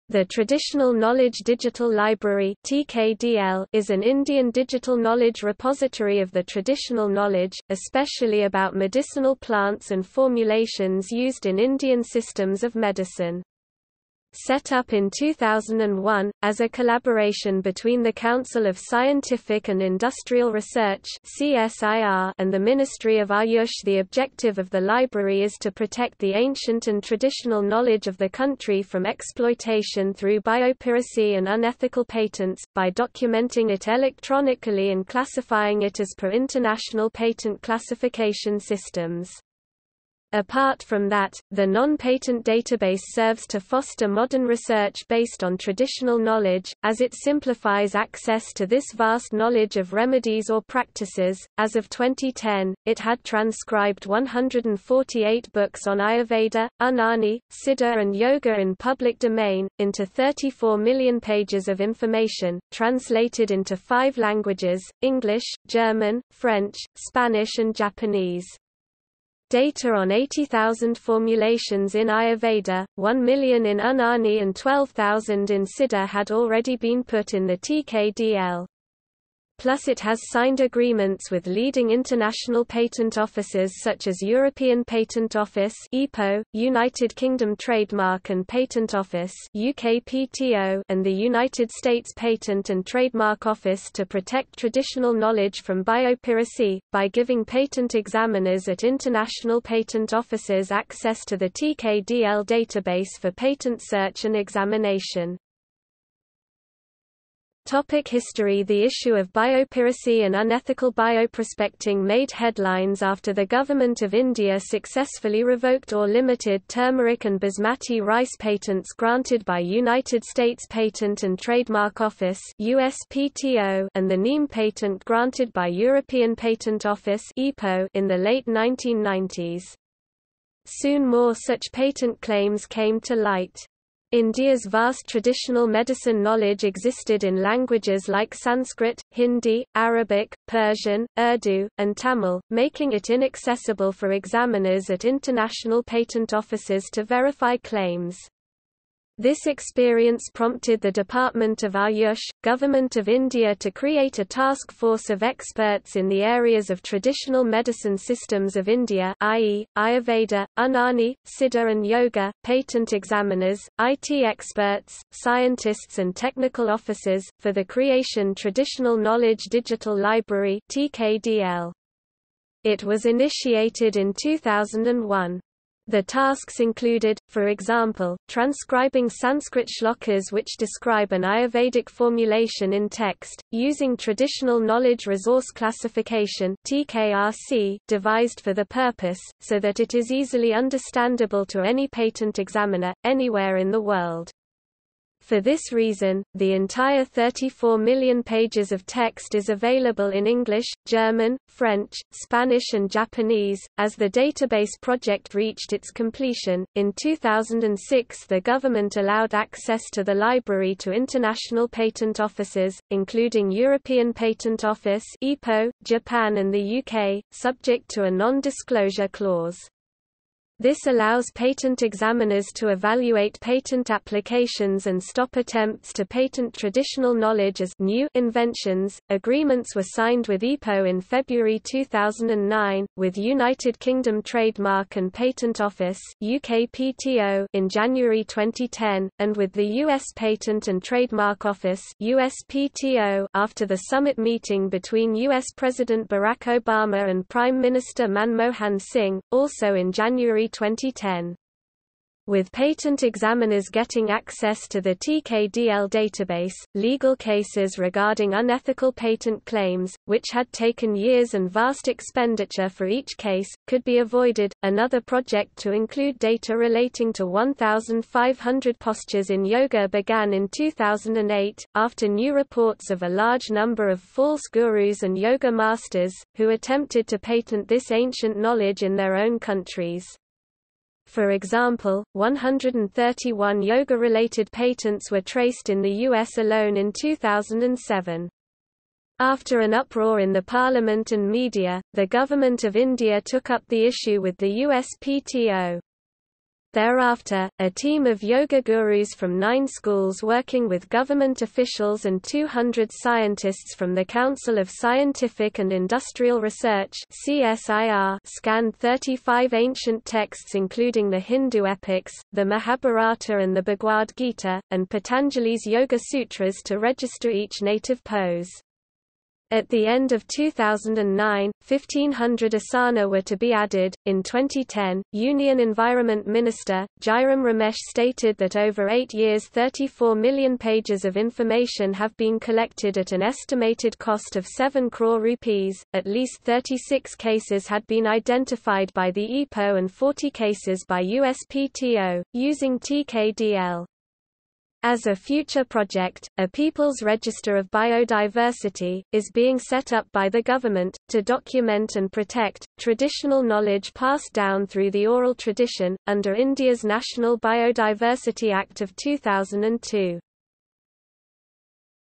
Traditional Knowledge Digital Library | Wikipedia audio article
00:01:47 History

- Socrates

SUMMARY
The Traditional Knowledge Digital Library (TKDL) is an Indian digital knowledge repository of the traditional knowledge, especially about medicinal plants and formulations used in Indian systems of medicine. Set up in 2001, as a collaboration between the Council of Scientific and Industrial Research (CSIR) and the  MINISTRY OF AYUSH the objective of the library is to protect the ancient and traditional knowledge of the country from exploitation through biopiracy and unethical patents, by documenting it electronically and classifying it as per international patent classification systems. Apart from that, the non-patent database serves to foster modern research based on traditional knowledge, as it simplifies access to this vast knowledge of remedies or practices.As of 2010, it had transcribed 148 books on Ayurveda, Unani, Siddha and Yoga in public domain, into 34 million pages of information, translated into five languages — English, German, French, Spanish and Japanese. Data on 80,000 formulations in Ayurveda, 1,000,000 in Unani and 12,000 in Siddha had already been put in the TKDL. Plus it has signed agreements with leading international patent offices such as European Patent Office (EPO), United Kingdom Trademark & Patent Office (UKPTO) and the United States Patent and Trademark Office to protect traditional knowledge from biopiracy, by giving patent examiners at international patent offices access to the TKDL database for patent search and examination.